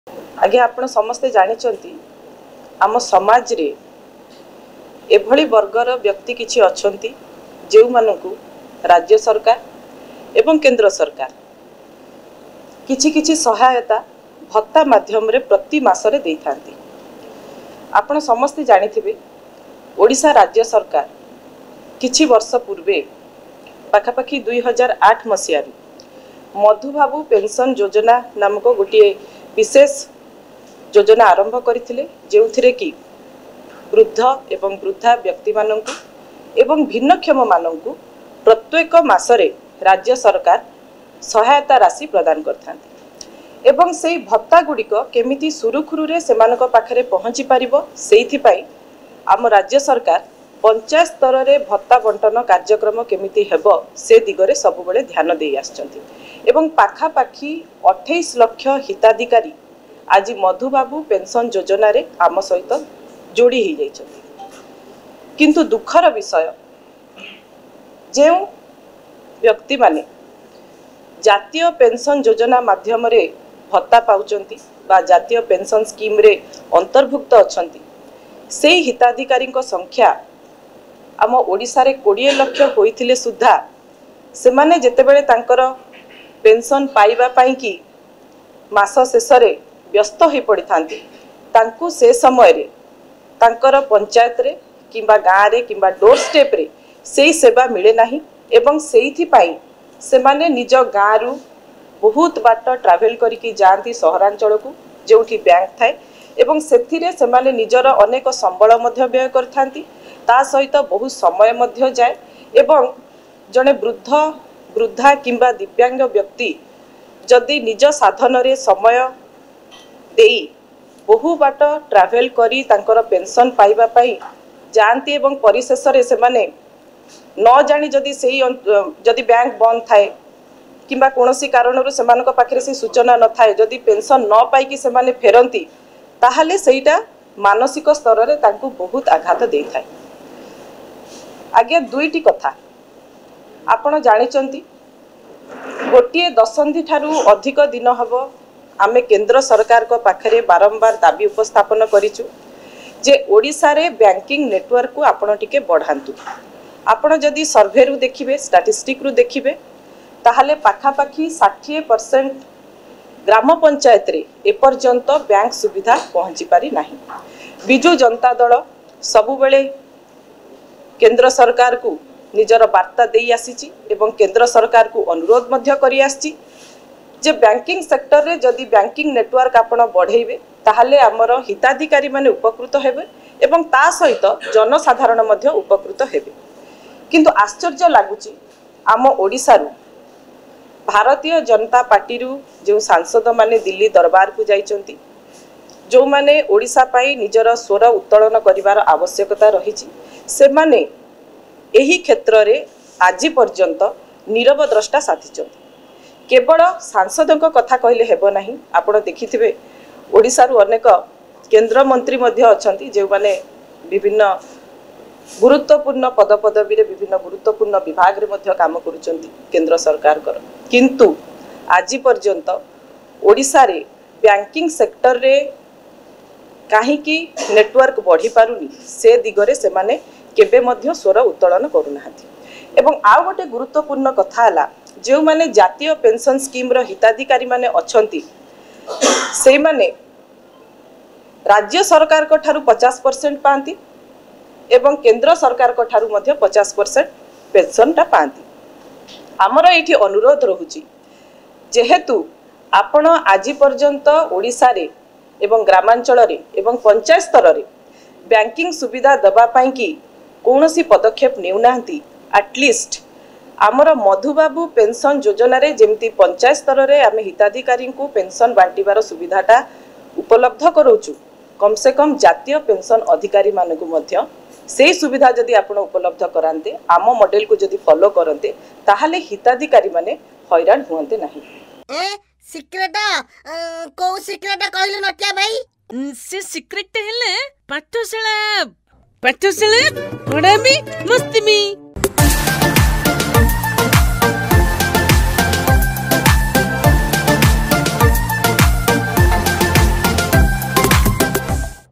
କେନ୍ଦ୍ର ସରକାର ଅବହେଳା ପାଇଁ ବରିଷ୍ଟ ନାଗରିକଙ୍କୁ ପେନସନ ମିଳିବାରେ ହେଉଛି ଅସୁବିଧା : ଶ୍ରିମୟି ମିଶ୍ର। PratidinTv
କେନ୍ଦ୍ର ସରକାର ଅବହେଳା ପାଇଁ ବରିଷ୍ଟ ନାଗରିକଙ୍କୁ ପେନସନ ମିଳିବାରେ ହେଉଛି ଅସୁବିଧା : ଶ୍ରିମୟି ମିଶ୍ର।